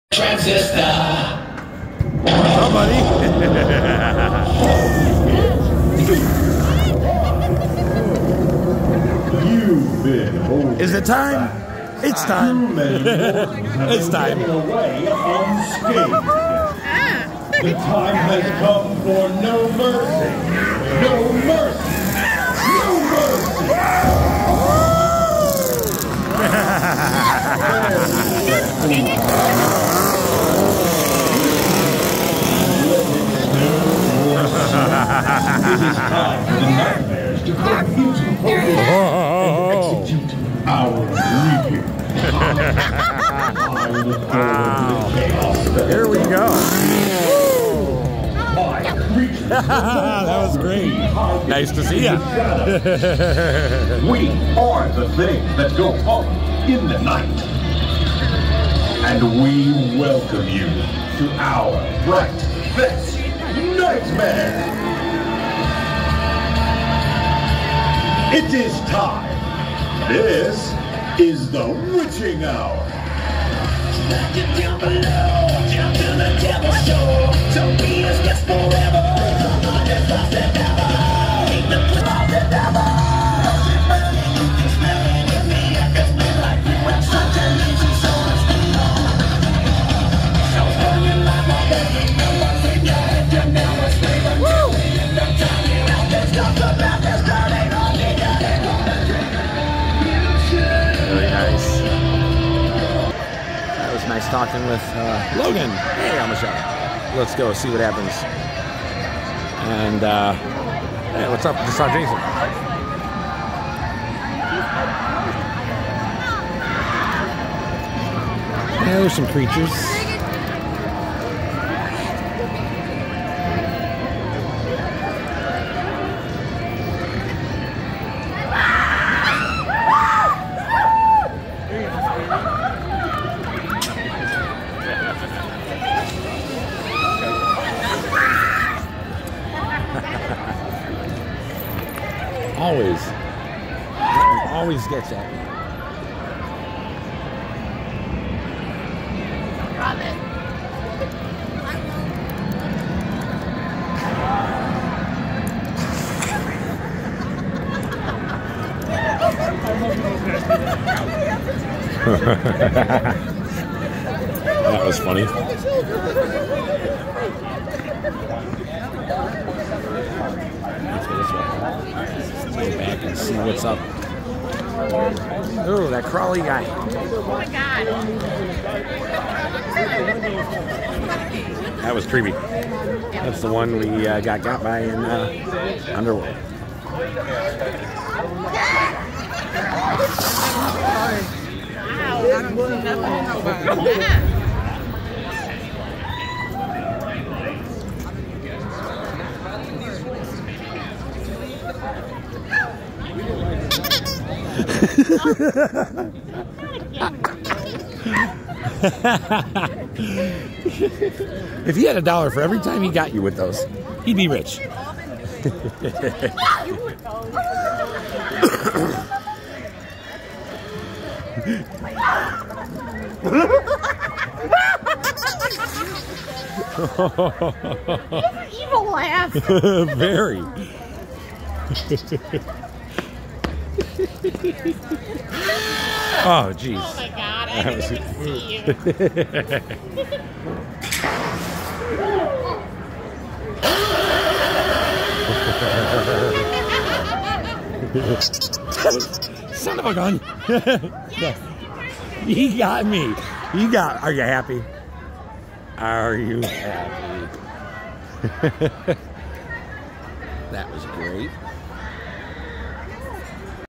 The Witching Hour Begins Pt.1 Fright Fest Six Flags Great America 11-1-24
Here comes the beginning of the wildest part of Fright Fest, The Witching Hour! My friend Logan found me as the fun began, but we soon got separated as L. Michelle and I blended into the madness! There was a girl who got scared and ran off, and seconds later I heard a scream of "What the F?!" One of the henchmen, Snapz, got L. Michelle before coming back to get me when I wasn't expecting it! Are you happy! Are ya?!